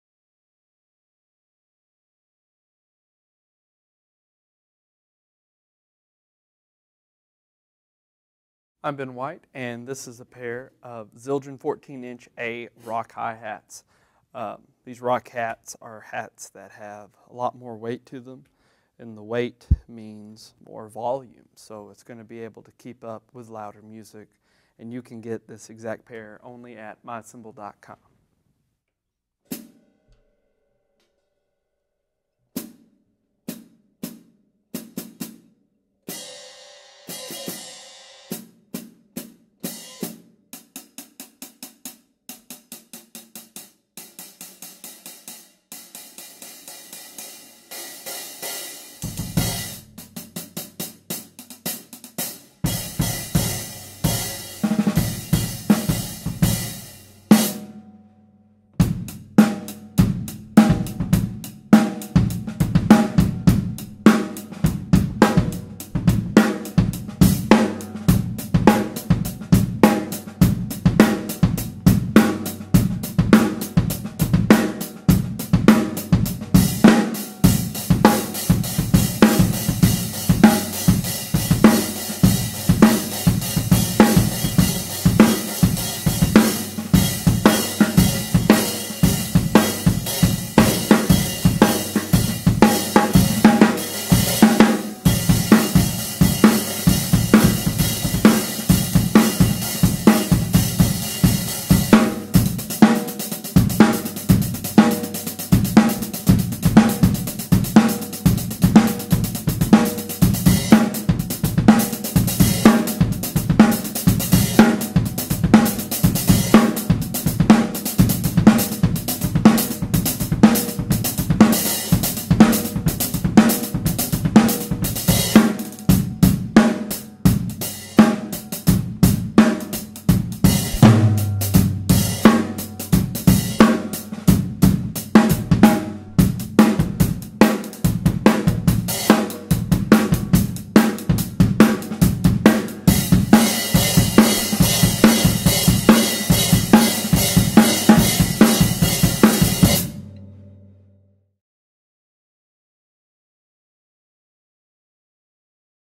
Zildjian 14" A Rock Hi Hat Cymbals (A0160-1071913B)
*SOLD* Zildjian 14" A Rock Hi Hat Cymbals.

Other gear used in this video:
• Cymbals (From drummers L to R):  Zildjian 20" A Crash Ride Cymbal, Zildjian 24" A Medium Ride Cymbal